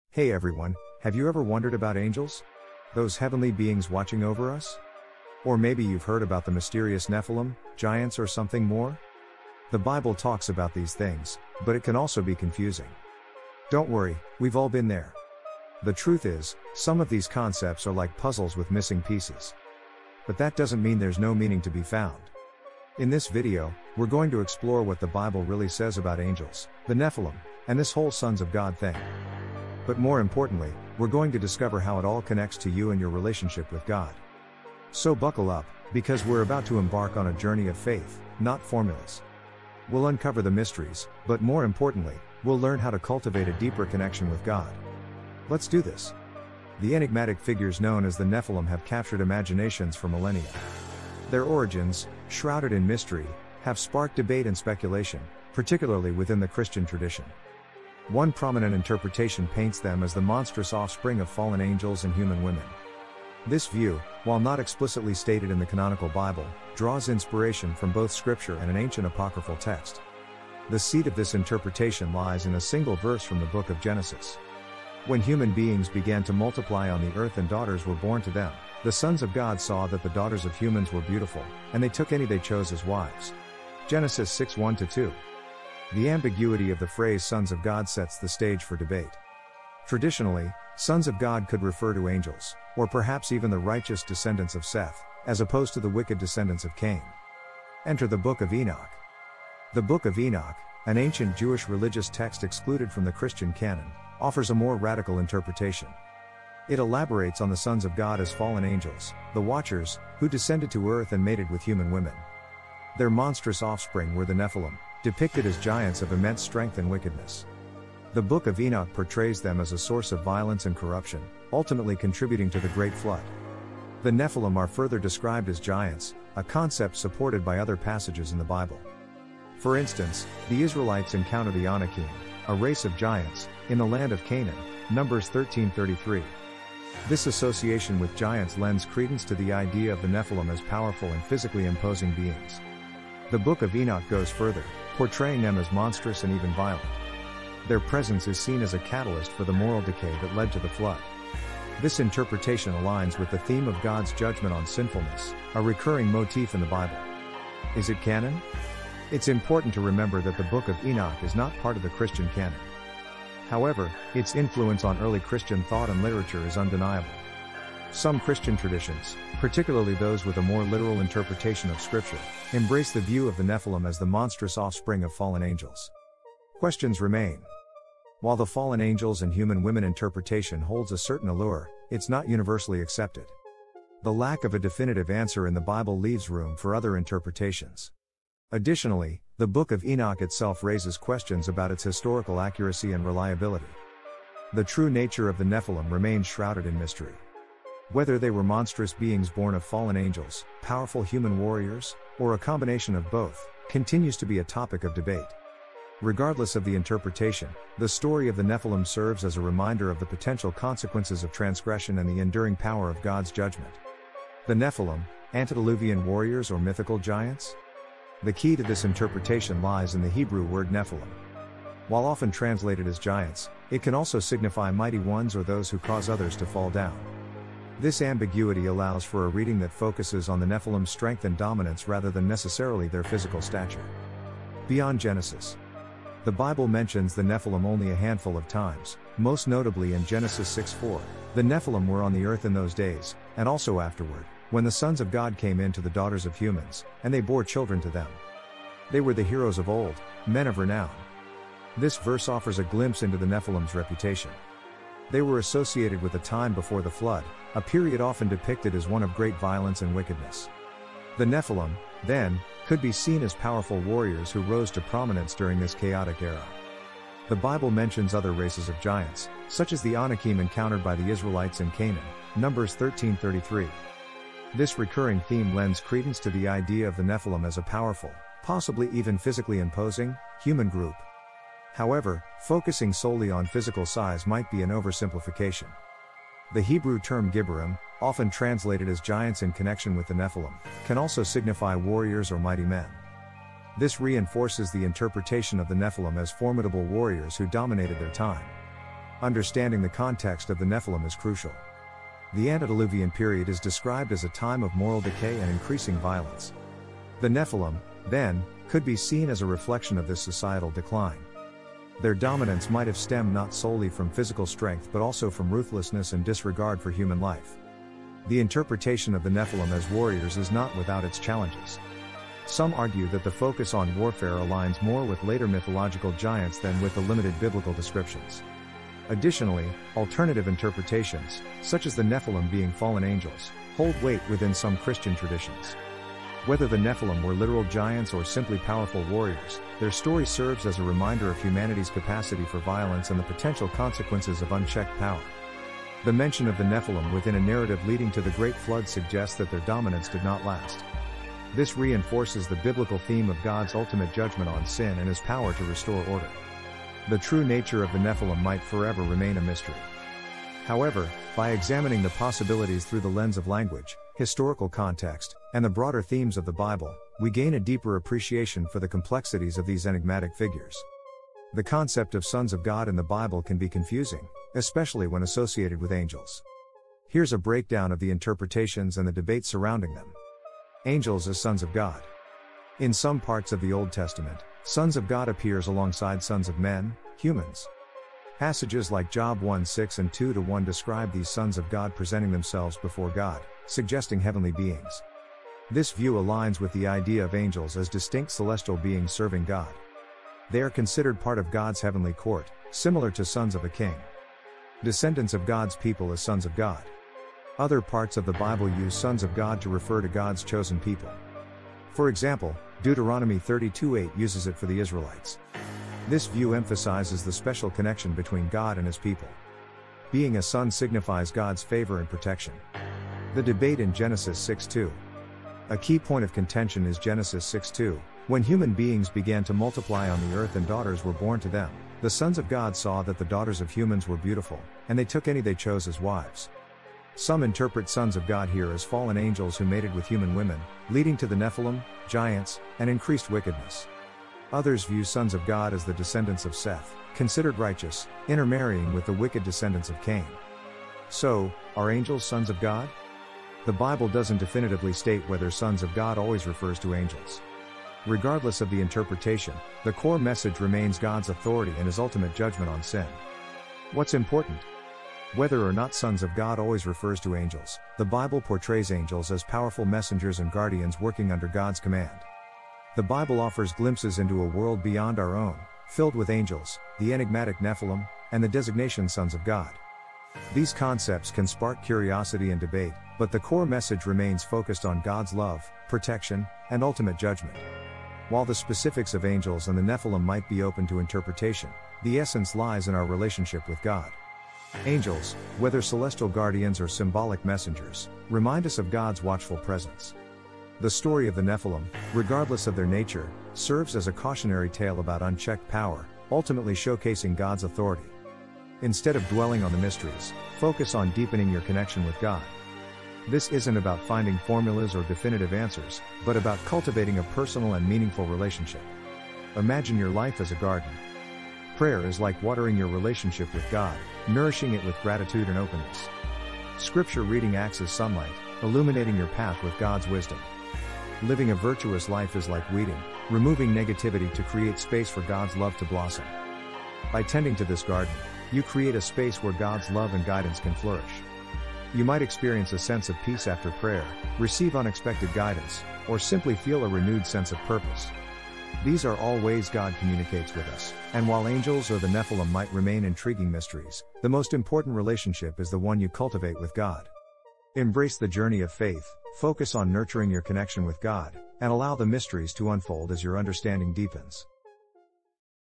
Nephilim, Demons, and Angels | What's the Difference?
Confused about angels? You're not alone! Some believe angels are the Nephilim, powerful beings from ancient myths. But is that true?

This video dives deep to clarify the difference! We'll explore the origins of the Nephilim story and contrast them with the benevolent angels we know and love. Discover why these powerful beings from the past are NOT your guardian angels.

Worried about who's watching over you? We'll clear the confusion and offer a message of hope about the true nature of angels – celestial protectors sent to guide you.